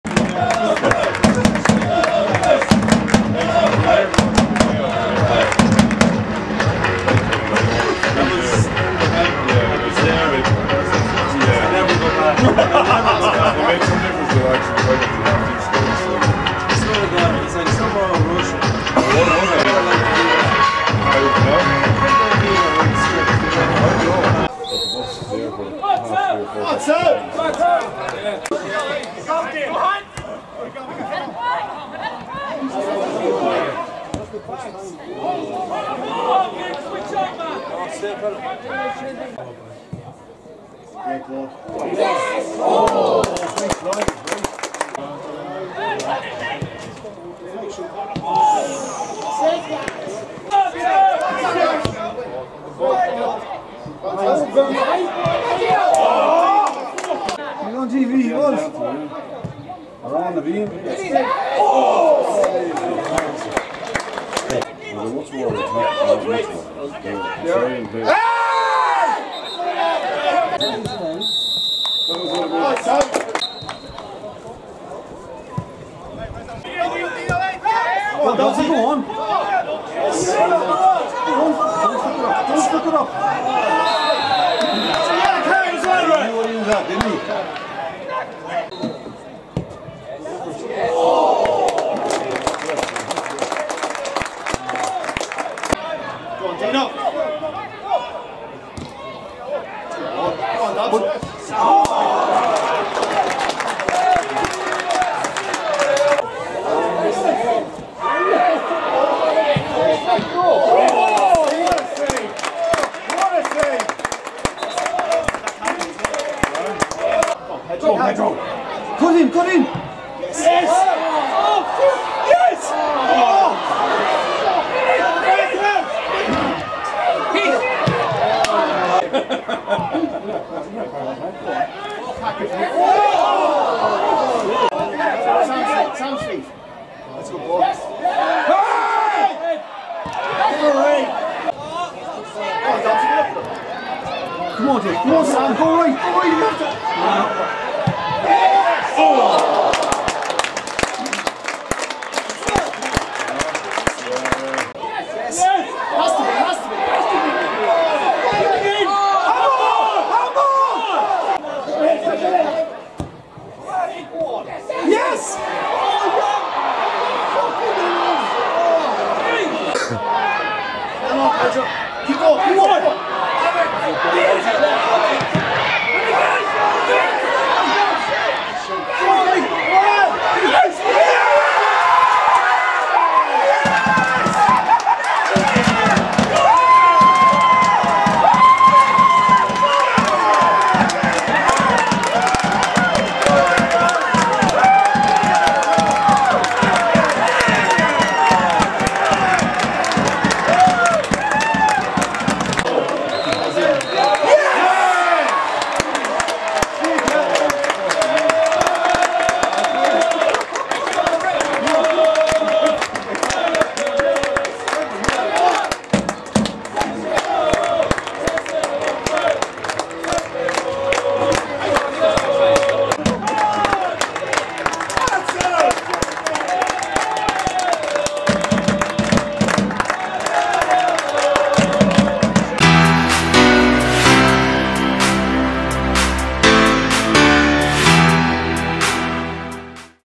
Longford Town FC v Wexford FC Highlights 08/06/19 | SSE Airtricity League
SSE Airtricity League First Division
Longford Town FC 1-0 Wexford FC
Saturday 8th June 2019,
City Calling Stadium, KO: 7:30PM